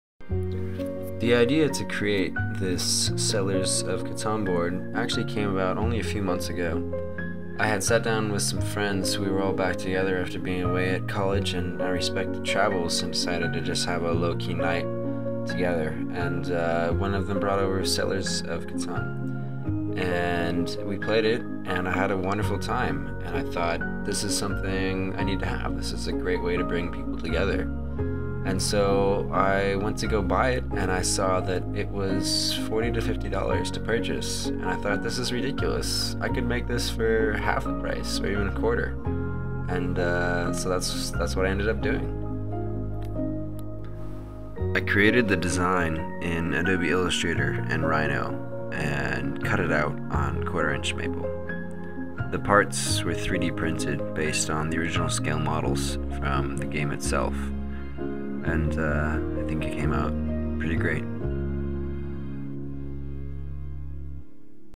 HCDE 498 Final Project: Settlers of Catan
Laser cut + 3D printed custom version of Settlers of Catan created for my final project in HCDE 498.